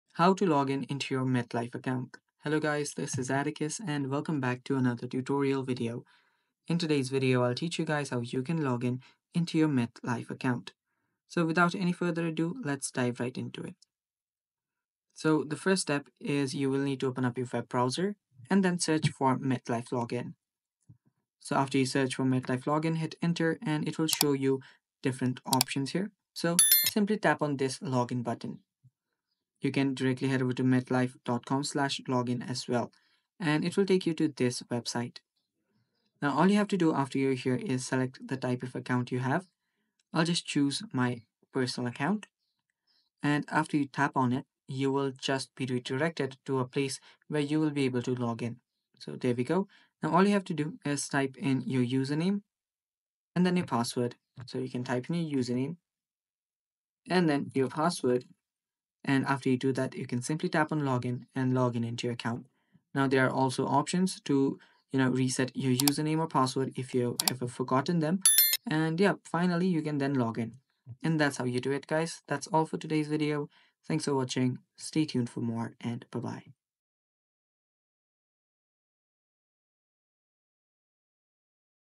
Metlife Login - How to Sign in to Metlife Provider Account (2025)
Need help accessing your Metlife Provider account? 🖥️🔐 This quick and easy 2025 guide will show you exactly how to log in to your Metlife Provider account step-by-step — fast, secure, and hassle-free.

What You’ll Learn in This Video:
✅ How to access the official Metlife Provider login page
✅ Step-by-step instructions to sign in successfully
✅ Common login issues and how to fix them
✅ Tips to reset your password or recover your username

Whether you're a dental provider, healthcare professional, or administrator, this tutorial will help you access your Metlife dashboard quickly and securely.

📌 Chapters:
⏱ 0:00 - Introduction
⏱ 0:10 - How to Sign in to Metlife Provider Account